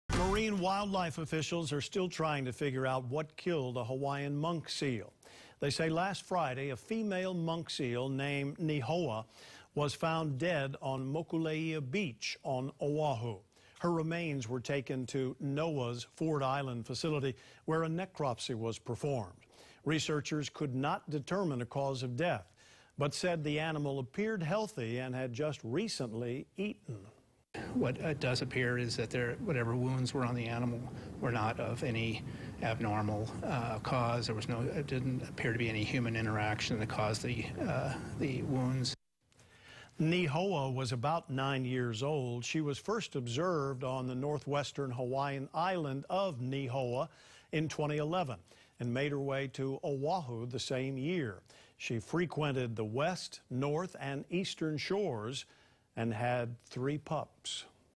Female Hawaiian monk seal found dead in Mokuleia
Researchers could not determine a cause of death, but say the animal was “in robust nutritional condition” and had just recently eaten.